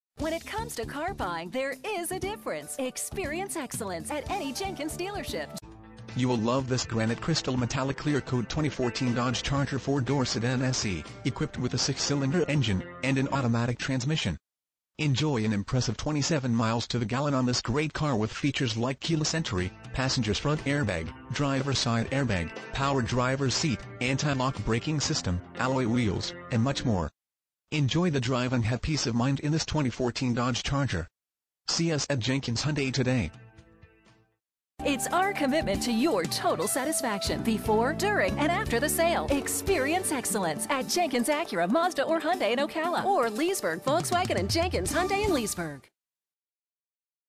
2014 Dodge Charger JENKINS HYUNDAI Ocala, FL 34474
Dodge Charger Imagine driving this  Granite Crystal Metallic Clear Coat   Dodge Charger 4 Dr Sedan, equipped with a 6 Cylinder engine and an automatic transmission. Enjoy an impressive 27 miles to the gallon on this great car with features like Keyless Entry, Passenger's Front Airbag, Driver Side Air Bag, Power Driver's Seat, Anti-Lock Braking System, Alloy Wheels, and much more. Enjoy the drive and have peace of mind in this  Dodge Charger. See us at JENKINS HYUNDAI today!
MILES:  9,956
VIN:2C3CDXBG9EH174466

JENKINS HYUNDAI
1602 SW College Rd.